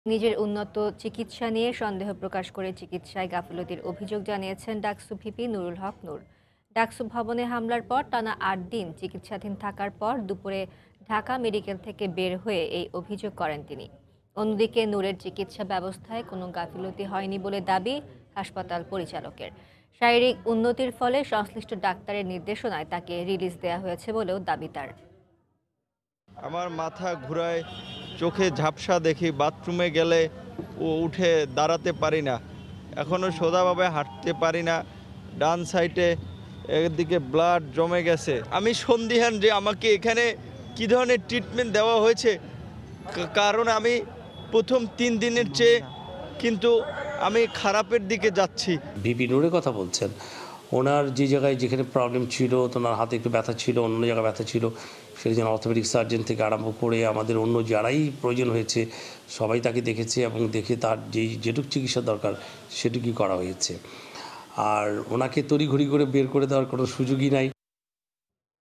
ঢাকা মেডিকেলে আমার সঠিক চিকিৎসা হয়নি | Nurul Haq Nur | Somoy TV
ঢাকা মেডিকেলে নিজের উন্নত চিকিৎসা নিয়ে সন্দেহ প্রকাশ করে চিকিৎসায় গাফিলতির অভিযোগ জানিয়েছেন ডাকসু ভিপি নুরুল হক নুর। ডাকসু ভবনে হামলার পর টানা ৮দিন চিকিৎসাধীন থাকার পর দুপুরে ঢাকা মেডিকেল থেকে বের হয়ে এই অভিযোগ করেন তিনি। অন্যদিকে নূরের চিকিৎসা ব্যবস্থায় কোন গাফিলতি হয়নি বলে দাবি হাসপাতাল পরিচালকের। শারীরিক উন্নতির ফলে সংশ্লিষ্ট ডাক্তারের নির্দেশনায় তাকে রিলিজ দেয়া হয়েছে বলেও দাবি তার।